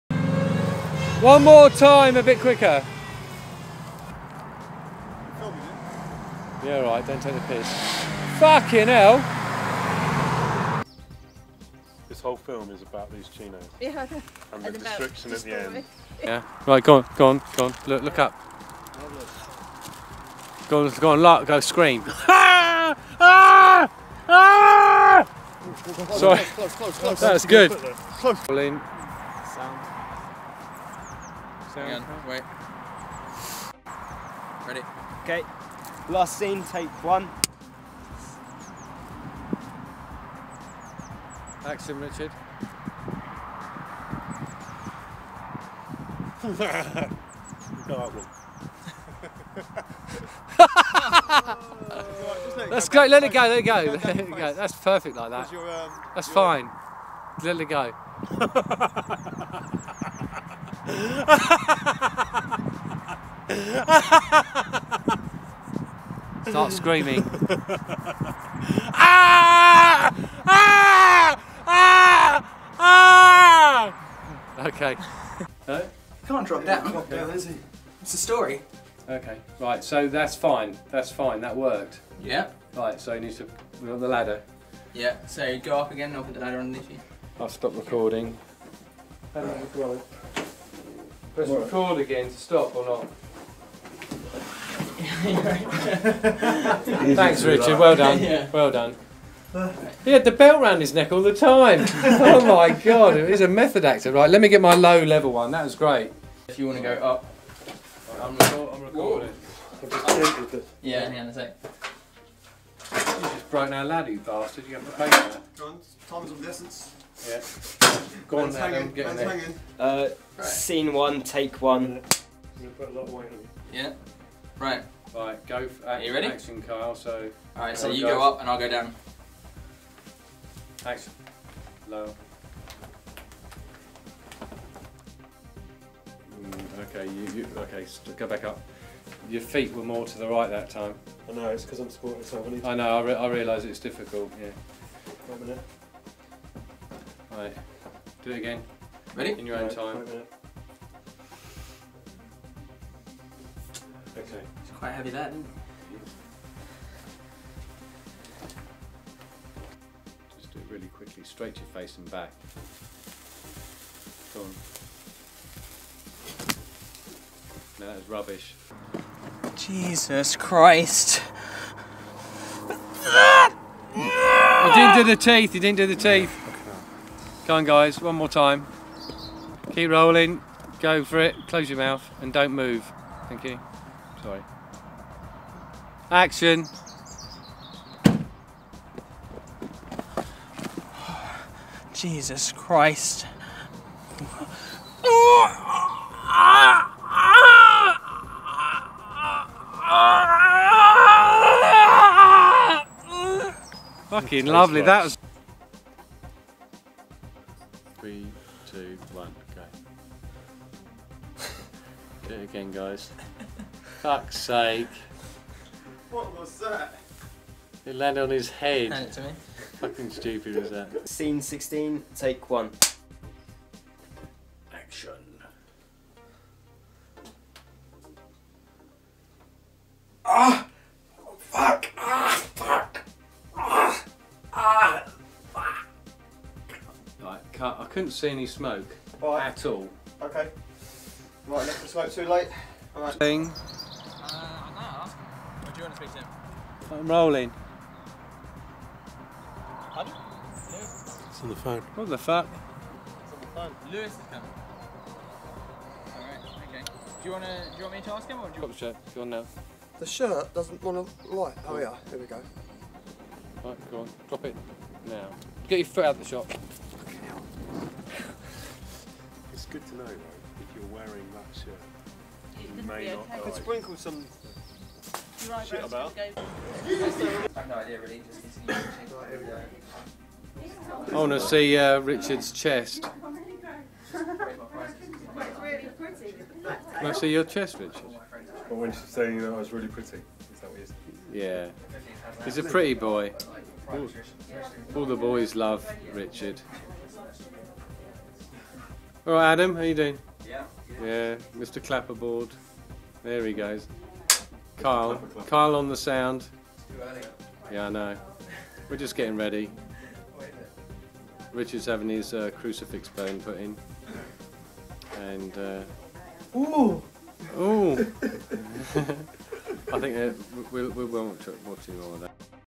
Behind the Scenes - Six Feet Under - a Mike Tack Film
Enjoy the laughter pain and frustration in this raw behind the scenes look at Apocalyptic Conservatory Studios entry for the 6 Short Cuts From Hell Competition SIX FEET UNDER starring the evergreen Richard Nock being subjected to all manner of physical and mental abuse from Director Mike Tack. Crew: Kyle Parke - Sound Tim Richards - Makeup  Adam Rabbitts - Clapper  Louise - Location Services